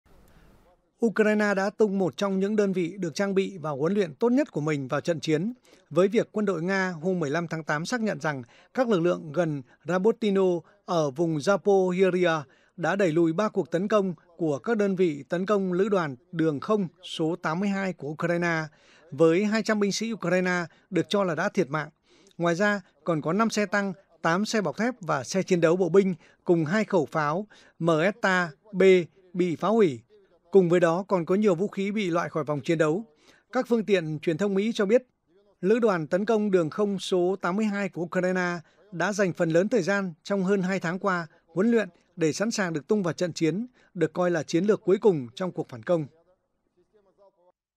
Ukraine tung đơn vị mạnh nhất vào trận giữa lúc nước sôi lửa bỏng | Tin Nga-Ukraine mới nhất
Lực lượng tấn công trên không của Ukraine cuối cùng đã triển khai đơn vị mạnh nhất. Lữ đoàn Tấn công trên không 82 với 2.000 binh lính, được trang bị các phương tiện chiến đấu Marder và Stryker cùng xe tăng Challenger 2, đã hành động ở Robotyne thuộc khu vực Zaporizhzhia ở phía Nam Ukraine trong một vài ngày qua.